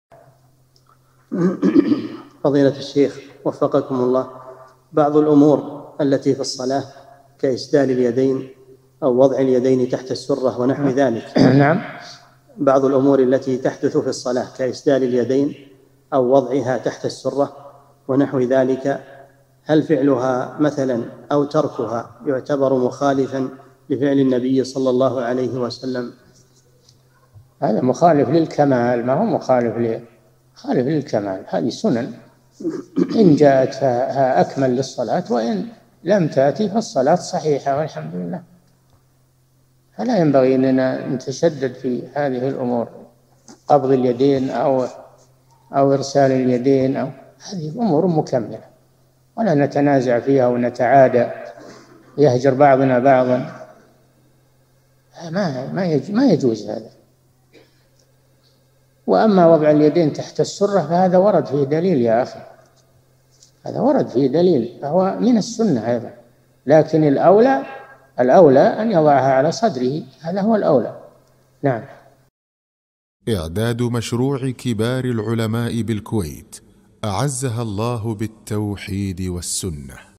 [11 -582] هل إسدال اليدين أو وضع اليدين تحت السرة من سنن الصلاة التي لا إثم على من تركها؟
-"السؤال: هل الأمور التي في الصلاة كإسدال اليدين أو وضع اليدين تحت السرة ونحو ذلك هل فعلها مثلًا أو تركها يعتبر مخالفًا لفعل النبي ﷺ؟!
الجَوَابُ: هذا مخالف للكمال، هذه سُنن إن جاءت فأكمل الصلاة وإن لم تأت فالصلاة صحيحة والحمد لله، فلا ينبغي أن نتشدد في هذه الأمور، قبض اليدين أو إرسال اليدين هذه أمور مكملة ولا نتنازع فيها ونتعادى ويهجر بعضنا بعضًا، هذا ما يجوز هذا، وأما وضع اليدين تحت السرة فهذا ورد فيه دليل يا أخي، فهو من السُّنة أيضًا، لكن الأولى أن يضعها على صدره هذا هو الأولى.
للاشتراك في قناة الشيخ صالح الفوزان
سؤال وجواب من شرح صفة الصلاة ( 582 ) مقطع

للاستفسار والإقتراحات والدعم
رئيس الفريق
لمشروع كبار العلماء